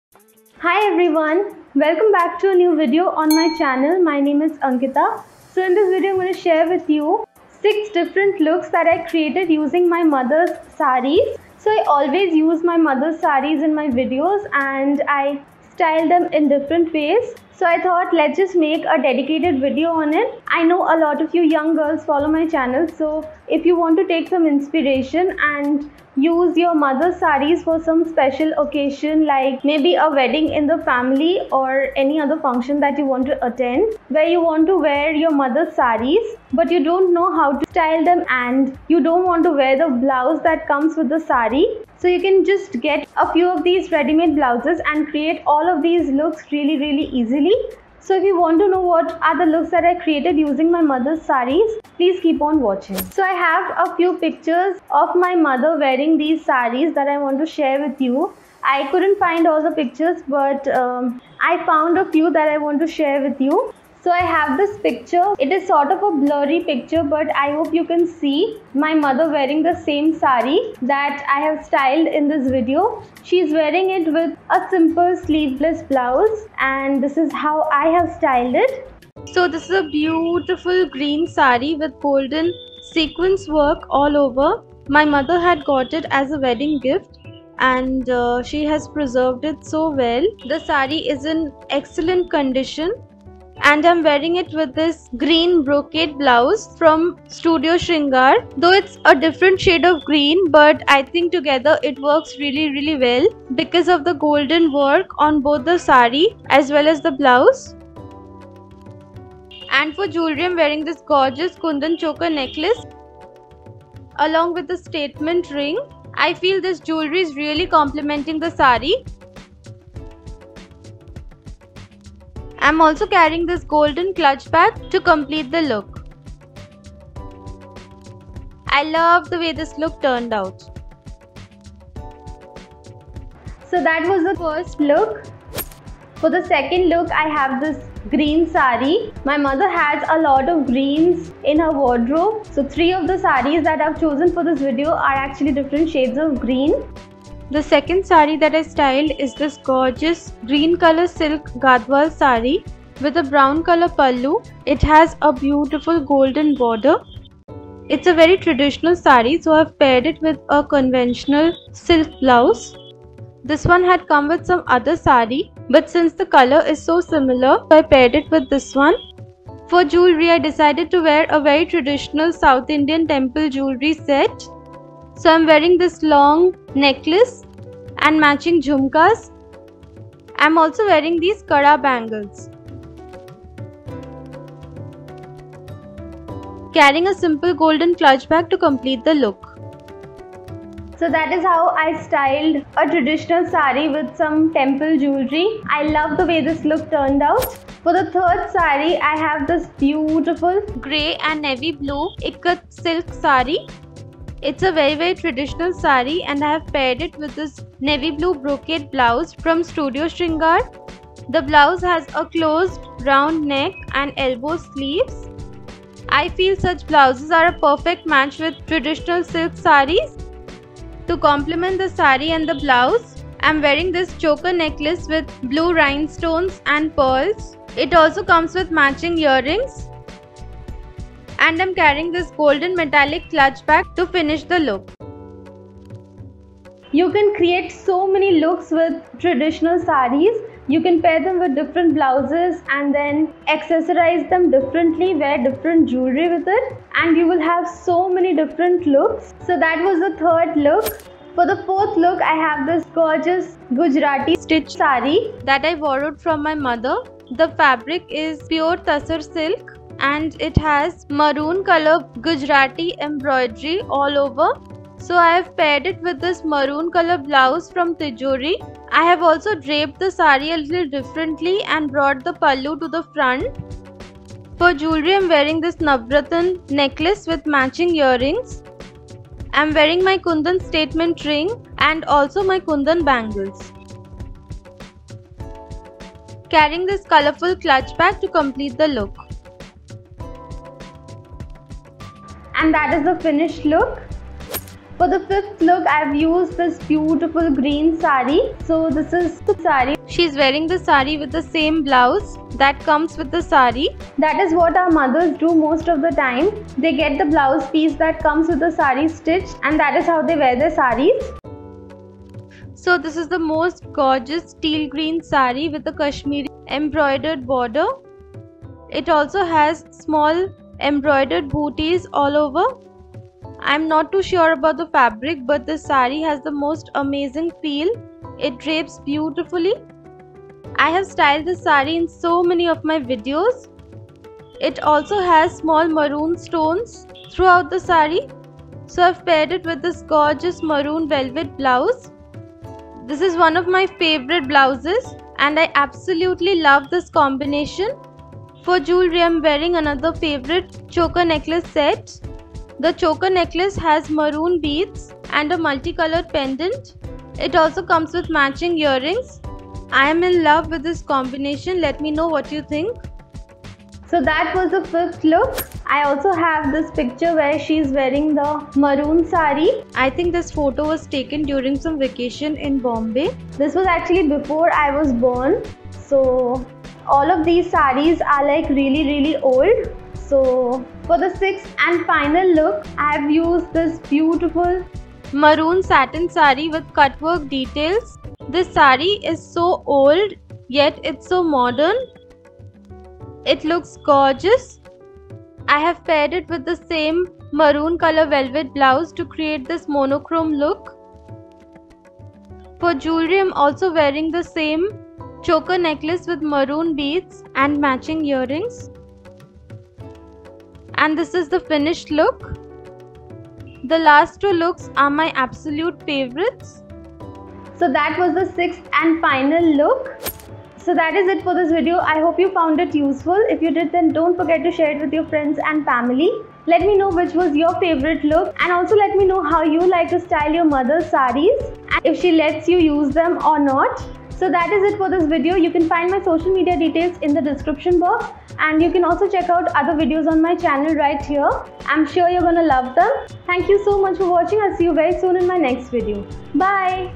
Restyling My Mother's Old Sarees | Saree lookbook
In this video, I will share with you, a few ways in which I Style my mother's old sarees.

Look 1:-

- Saree: Green Saree with Sequin Work
- Green Brocade Blouse
- Kundan Necklace (Similar)
- Kundan Ring (Similar)
- Golden Clutch Bag

Look 2:-

- Green Silk Gadwal Saree
- Green Blouse: Stitched
- Temple Jewelry (Similar)
- Bangles: Old
- Golden Clutch Bag: Old

Look 3:-

- Grey & Navy Blue Ikat Silk Saree
- Navy Blue Brocade Blouse
- Blue Choker Necklace Set
- Golden Clutch Bag

Look 4:-

- Navratna Jewelry Set
- Kundan Ring (Similar)
- Multicolor Bag: old

Look 5:-

- Teal Green Saree with Kashmiri Embroidery
- Maroon Velvet Blouse

Look 6:-

- Maroon Satin Cutwork Saree
- Maroon Velvet Blouse

Equipment used:
Camera: Canon EOS 700D ( Click to buy
MIC: Rode VideoMicro Compact On-Camera Microphone ( Click to buy
Tripod: Similar one ( Click to buy